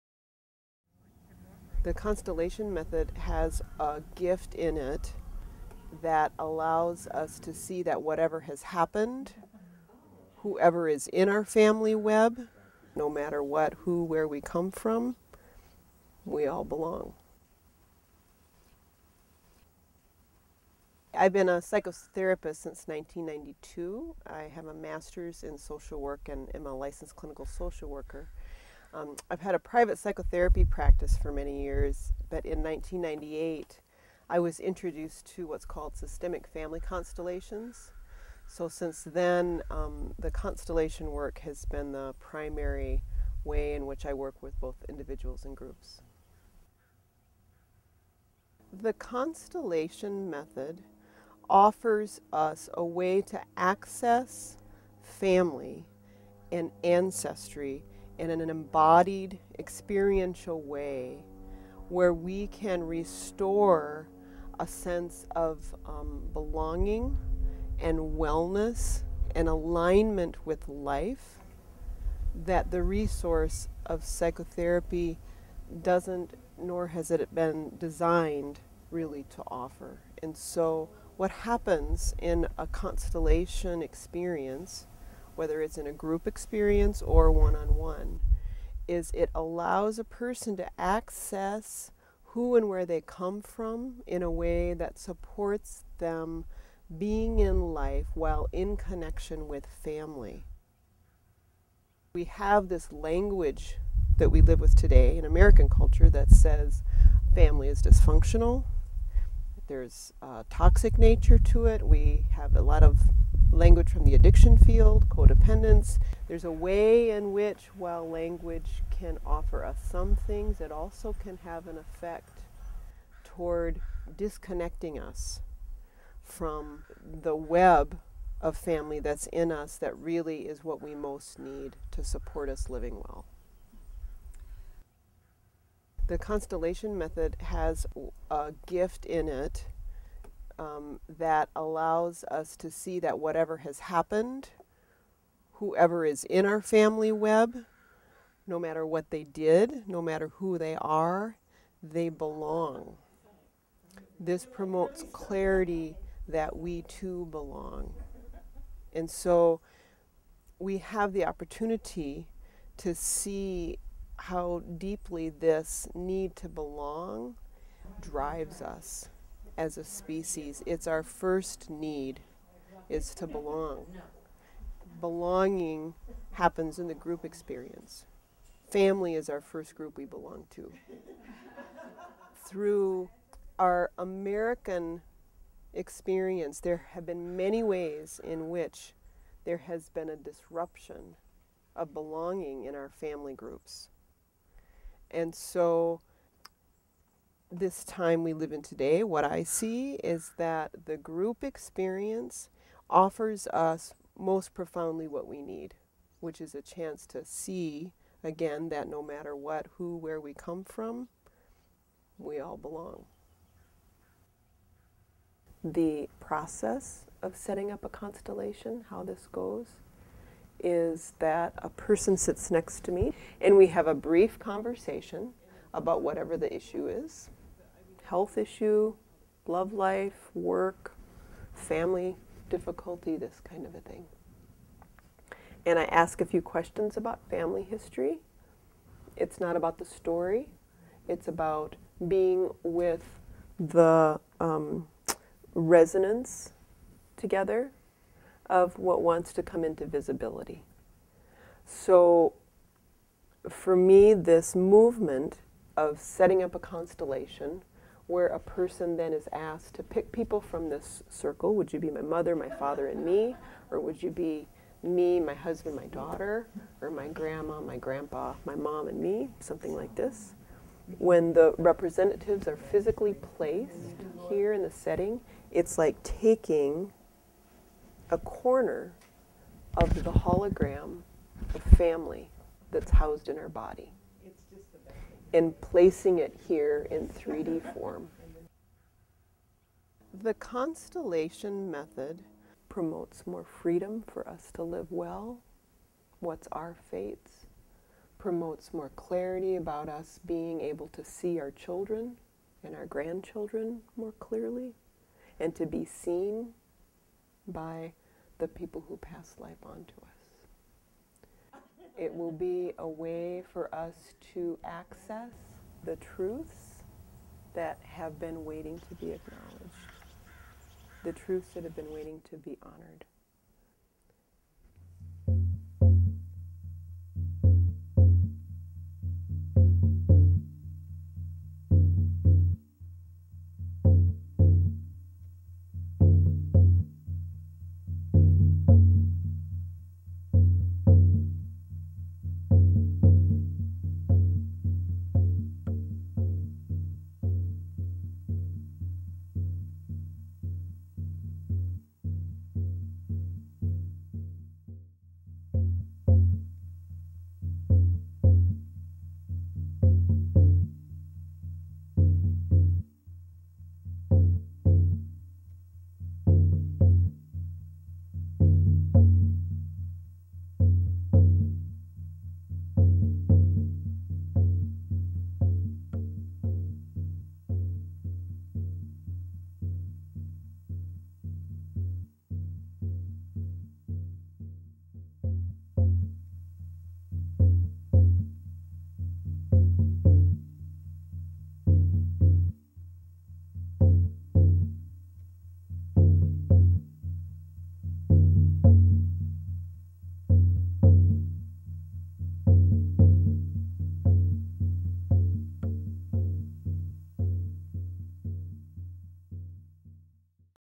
Lisa Iversen on the Systemic (Family) Constellation Method
Filmmaker Regan Johnson spent a day in August 2012 taping a Systemic Constellation circle led by Lisa Iversen. Regan produced four videos from that experience, shedding light on the connection between the Systemic Constellation method and healing the colonial past of the United States for all Americans today...and tomorrow.

Thank you to all those who participated in these videos, including Sabine and Dan Olsen, Kate Regan, Simona Draghici, and Richard Lemaire.